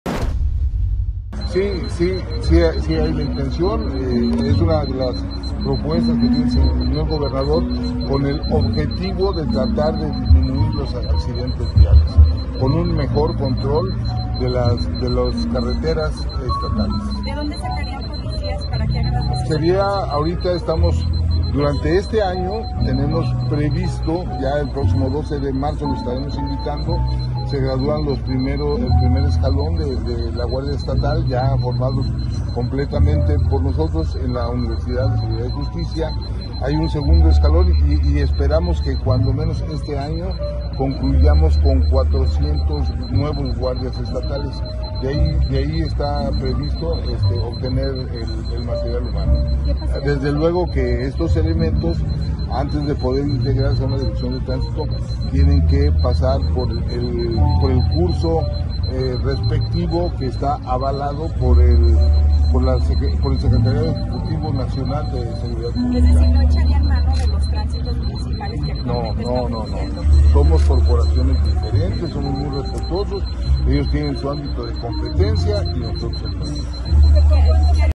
𝐓𝐑𝐀𝐍𝐒𝐈𝐓𝐎 𝐄𝐒𝐓𝐀𝐓𝐀𝐋
El secretario de seguridad pública de Tamaulipas, Sergio Hernando Chávez García, confirmó que se promovió una iniciativa ante el congreso del estado para crear la dirección estatal de tránsito, la cual tendría como ámbito de competencia resguardar las vías de comunicación estatales.